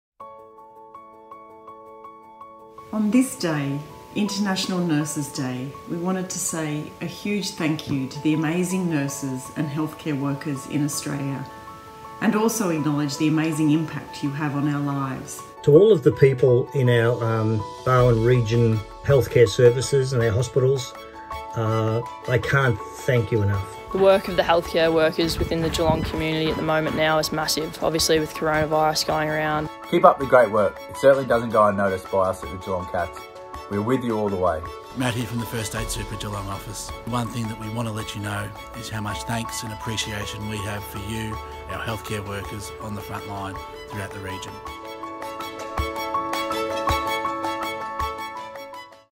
First State Super and the Geelong Cats say thank you to Australia’s Nurses
Today we’d like to acknowledge the incredible efforts of all nurses across Australia and the globe as we celebrate International Nurses Day.

As we recognise so many of our amazing members and their colleagues, we invite you to join us in thanking our nurses who've done so much, not only now but throughout our lives.

As a first step, our partners at the Geelong Cats have shared their thoughts on the important role nurses play in their community. Cats’ CEO Brian Cook, AFL star Mitch Duncan and AFLW midfielder Nina Morrison know how amazing nurses are.

Share with us some of the stories you have of the difference a nurse has made in your life!